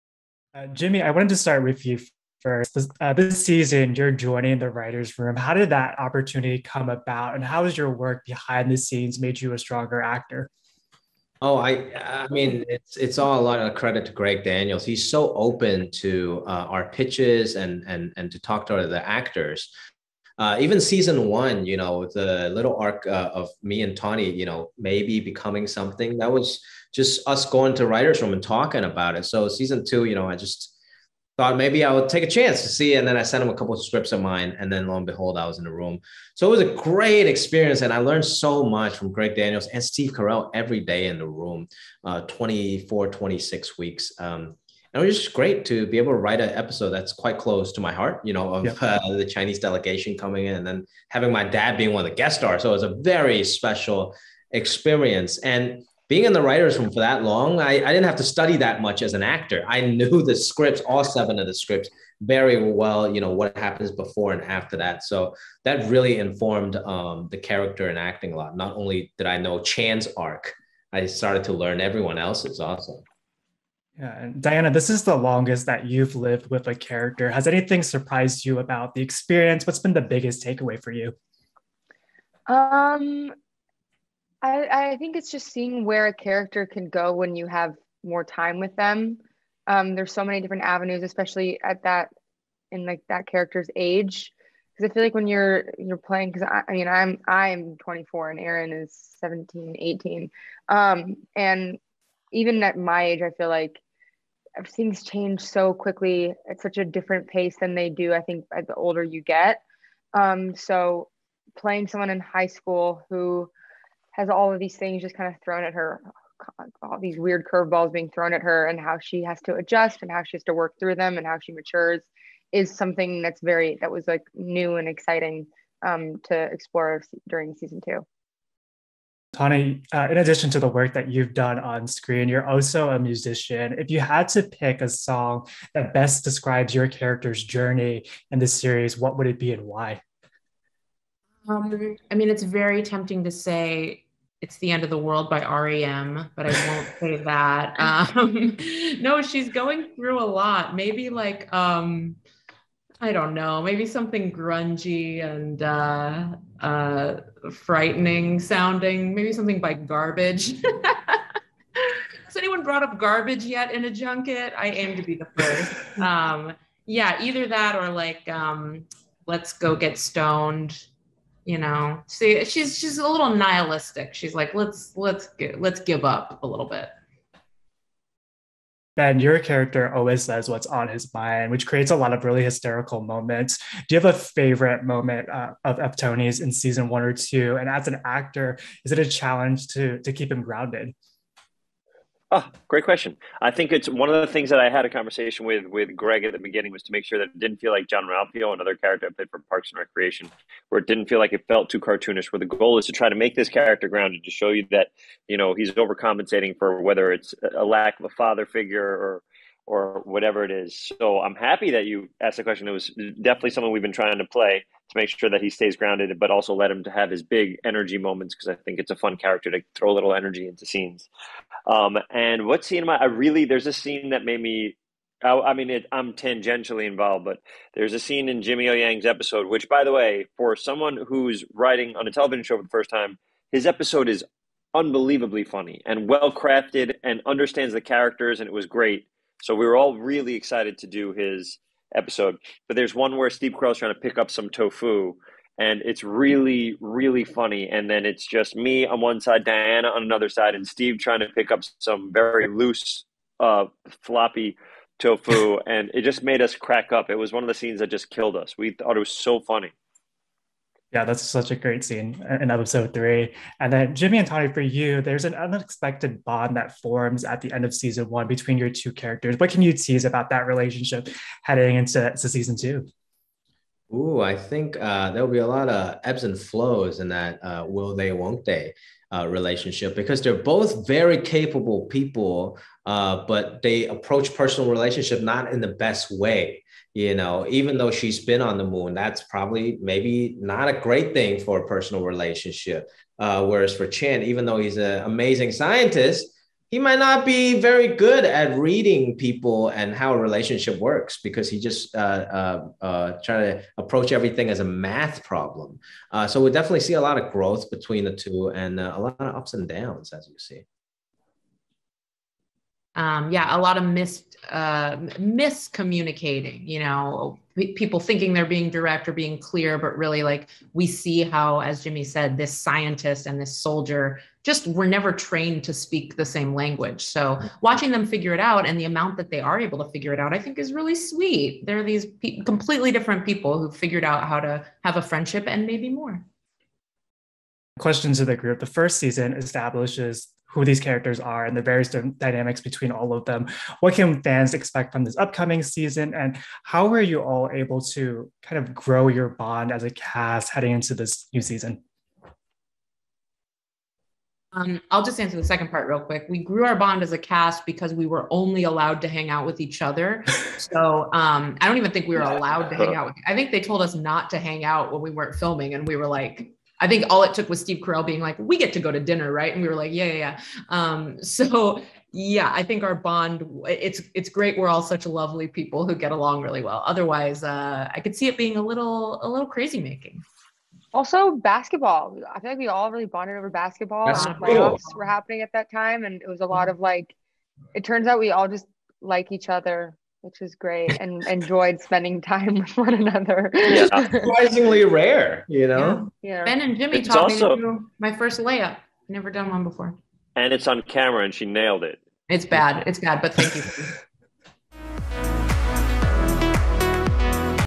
Ben Schwartz, Diana Silvers, Jimmy O. Yang, and Tawny Newsome Talk Space Force Season 2
About Space Force
Season 2 picks up with General Naird (Steve Carell) and his underdog team having to prove their worth to a new administration while dealing with interpersonal challenges. Will the group come together or fall apart under the pressure?